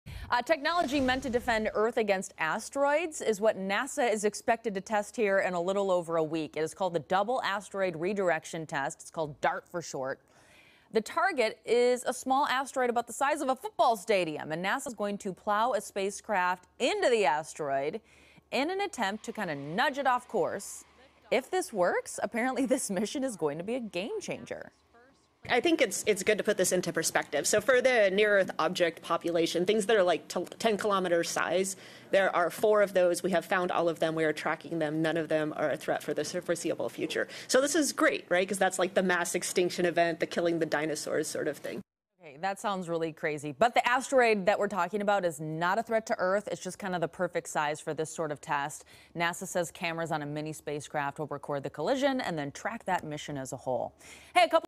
NASA to test asteroid defense system | FOX 13 Seattle
NASA is expected to test new technology meant to defend earth against asteroids.